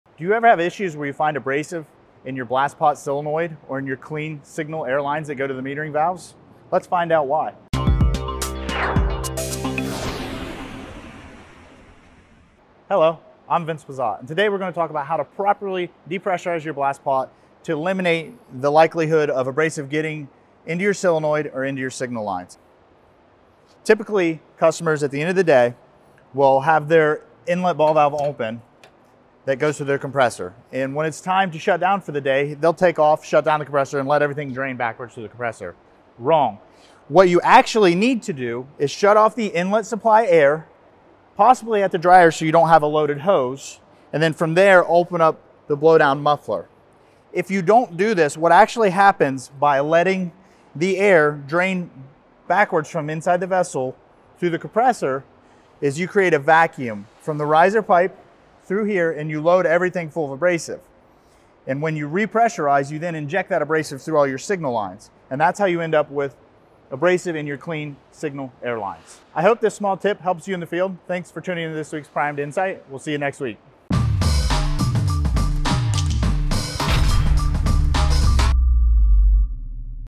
Preventing Abrasive From Entering Your Solenoid and Control Lines
In this Primed Insight Vince explains the proper way to depressurize your blast pot to prevent abrasive from entering the solenoid and clean signal airlines.